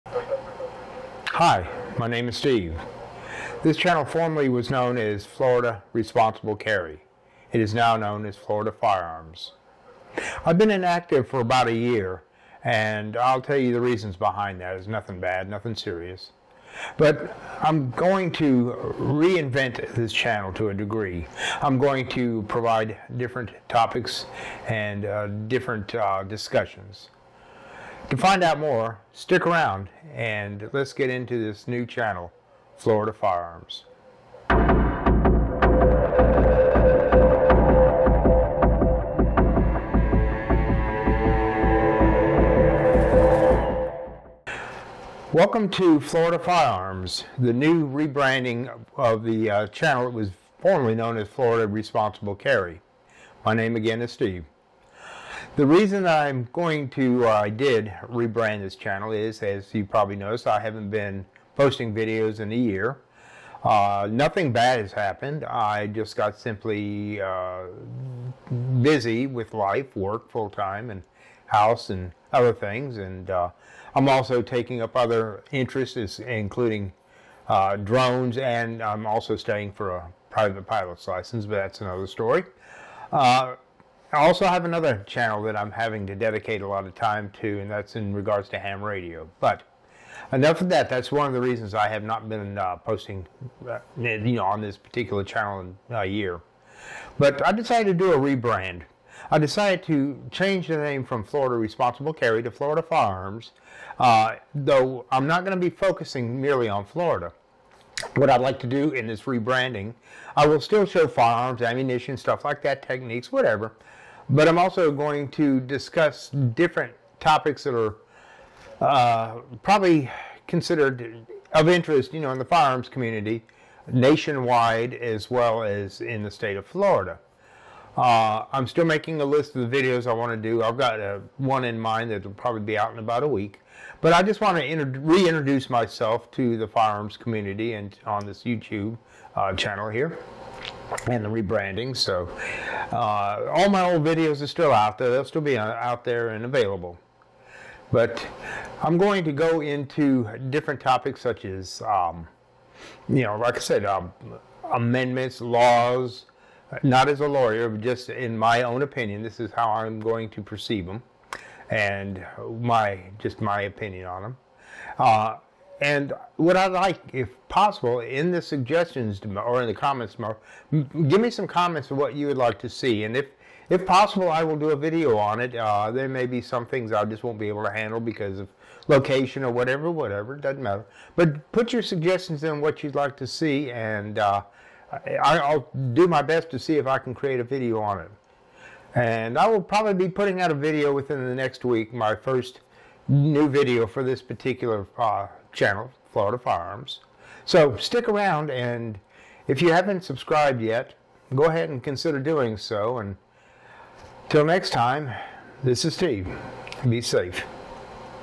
Rebranding - Florida Responsible Carry is now FLORIDA FIREARMS
Florida Responsible Carry has been dormant for a year.  This was due to burnout and lack of new material.  The channel has been rebranded and will offer up new videos along with a discussion on topics that are of interest to the firearms community.